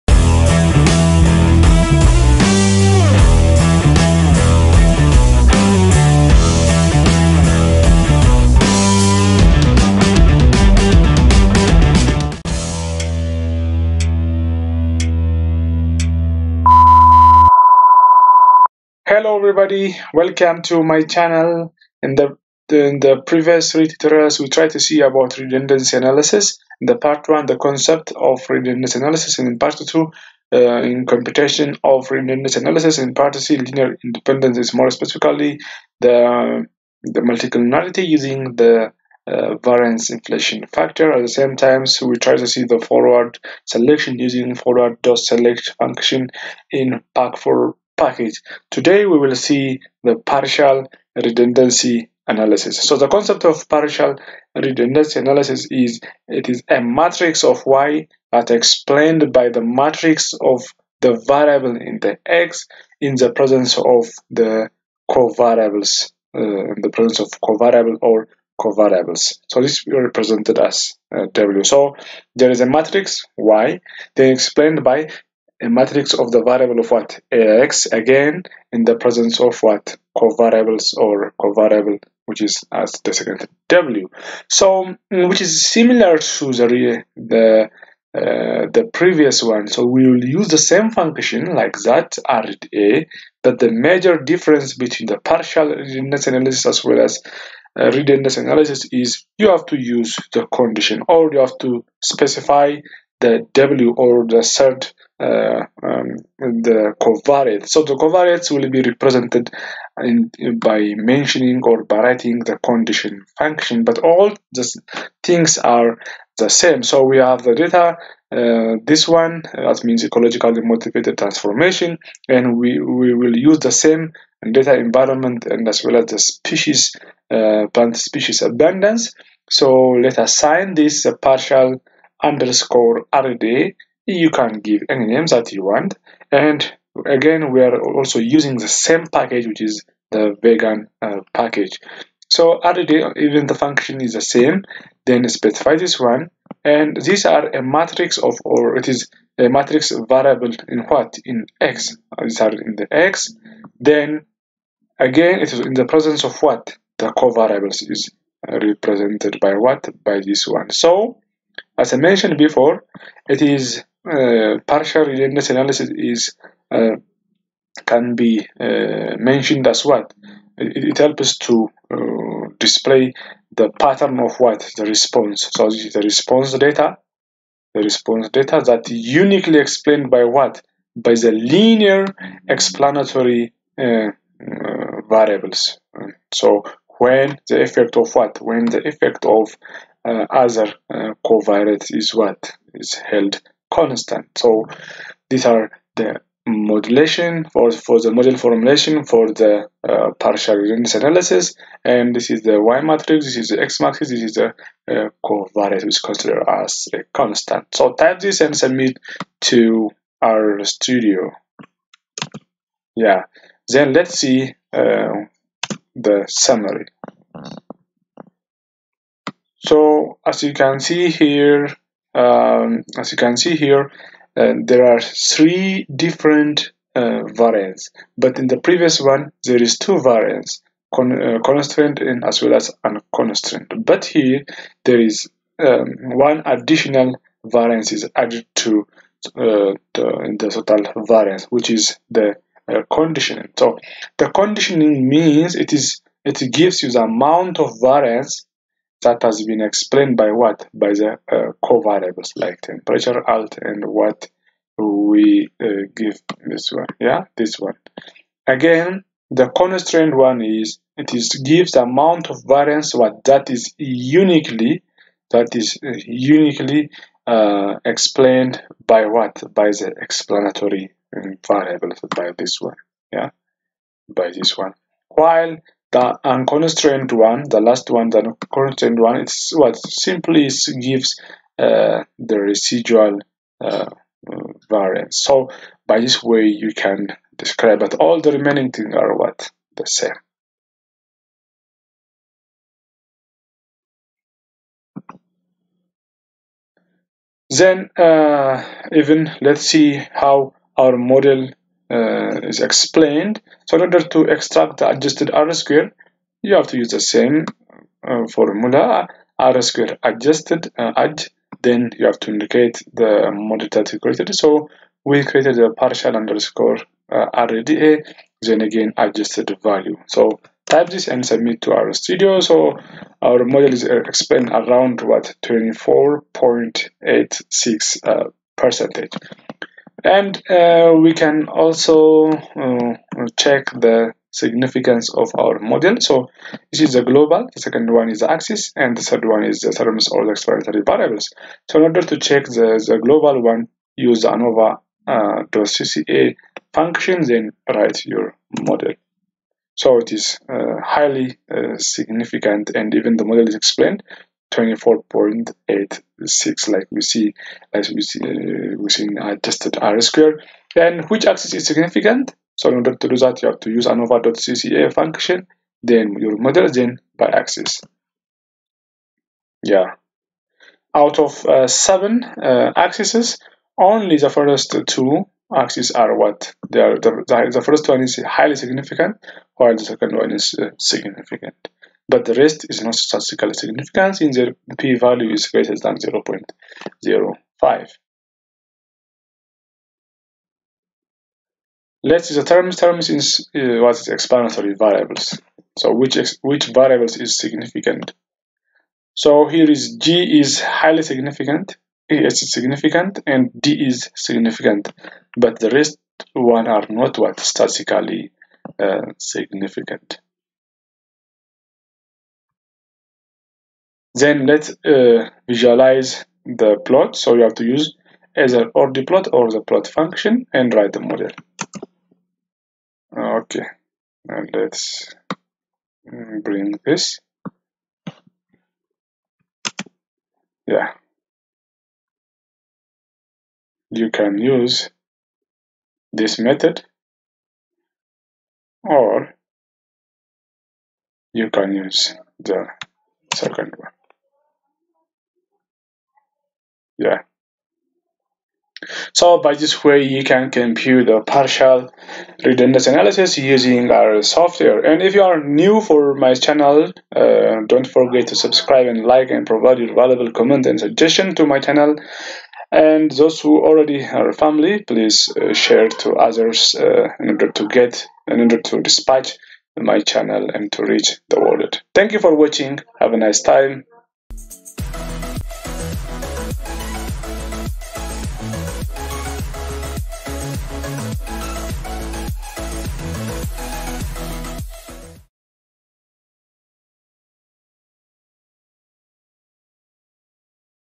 Partial redundancy analysis in R |part -4
Like other partial methods (e.g. partial CCA), partial redundancy analysis (pRDA) seeks to remove the effect of one or more explanatory variables on a set of response variables prior to a standard RDA. This may be useful when well-characterized variables with strong effects obscure the effects of more interesting explanatory variables.
MODEL FORMULATION
spe_hellinger =decostand(spe, method = "hellinger")
partial_rda =rda(spe_hellinger ~ Rn + G + es + ra + d +
                     zom + uz + Condition(Temp + alt + rl + zoh), data = env)
summary(partial_rda)
# model’s explanatory power?
Extract the model's adjusted R2
RsquareAdj(partial_rda)$adj.r.squared
Is the model statistically significant?